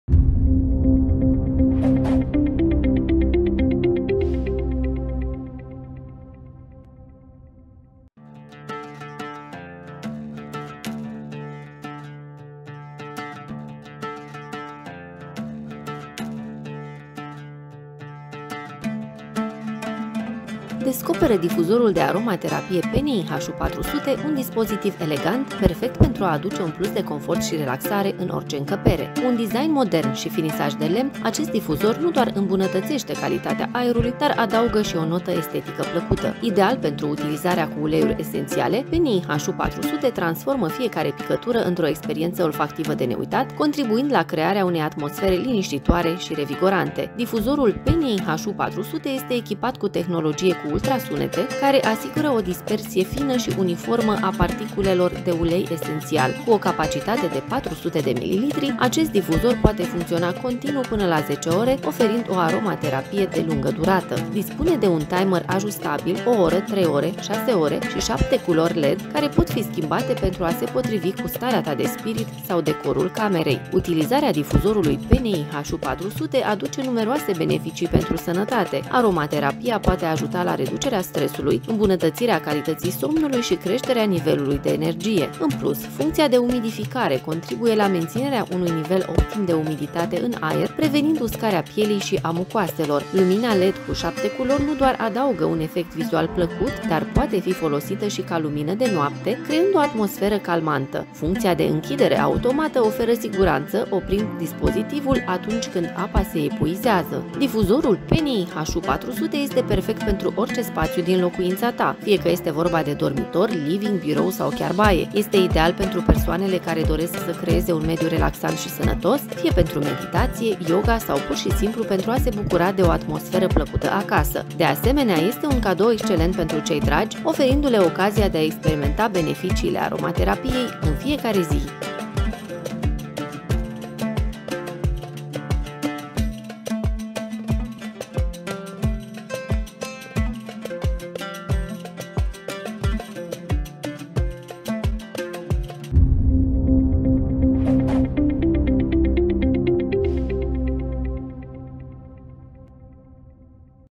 Difuzor aromaterapie PNI HU400 pentru uleiuri esentiale RO
Descoperă difuzorul aromaterapie PNI HU400, un dispozitiv elegant și perfect pentru a aduce un plus de confort și relaxare în orice încăpere. Cu un design modern și finisaj de lemn, acest difuzor nu doar că îmbunătățește calitatea aerului, dar adaugă și o notă estetică plăcută.
Ideal pentru utilizarea cu uleiuri esențiale, PNI HU400 transformă fiecare picătură într-o experiență olfactivă de neuitat, contribuind la crearea unei atmosfere liniștitoare și revigorante.
Difuzorul PNI HU400 este echipat cu tehnologie cu ultrasunete, care asigură o dispersie fină și uniformă a particulelor de ulei esențial. Cu o capacitate de 400 ml, acest difuzor poate funcționa continuu până la 10 ore, oferind o aromaterapie de lungă durată.
Dispune de un timer ajustabil (1h, 3h, 6h) și 7 culori LED care pot fi schimbate pentru a se potrivi cu starea ta de spirit sau decorul camerei.
Utilizarea difuzorului PNI HU400 aduce numeroase beneficii pentru sănătate. Aromaterapia poate ajuta la reducerea stresului, îmbunătățirea calității somnului și creșterea nivelului de energie.
În plus, funcția de umidificare contribuie la menținerea unui nivel optim de umiditate în aer, prevenind uscarea pielii și a mucoaselor. Lumina LED cu 7 culori nu doar că adaugă un efect vizual plăcut, dar poate fi folosită și ca lumină de noapte, creând o atmosferă calmantă.
Funcția de închidere automată asigură siguranța, oprind dispozitivul atunci când apa se epuizează
Difuzorul PNI HU400 este perfect pentru orice spațiu din locuința ta, fie că este vorba de dormitor, living, birou sau chiar baie. Este ideal pentru persoanele care doresc să creeze un mediu relaxant și sănătos, fie pentru meditație, yoga sau pur și simplu pentru a se bucura de o atmosferă plăcută acasă.
De asemenea, este un cadou excelent pentru cei dragi, oferindu-le ocazia de a experimenta beneficiile aromaterapiei în fiecare zi.